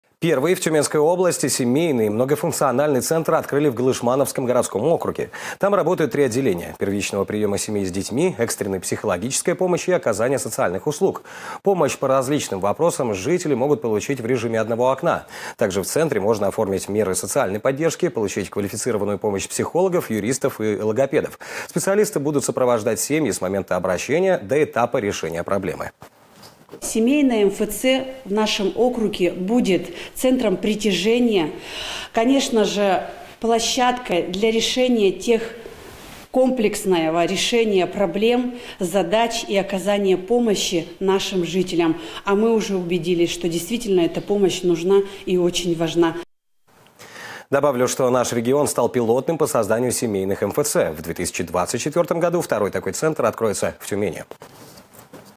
Для всей семьи
Первый в Тюменской области семейный многофункциональный центр открыли в Голышмановском городском округе. В центре можно оформить меры социальной поддержки, получить квалифицированную помощь психологов, юристов и логопедов. Специалисты будут сопровождать семьи с момента обращения до этапа решения проблемы.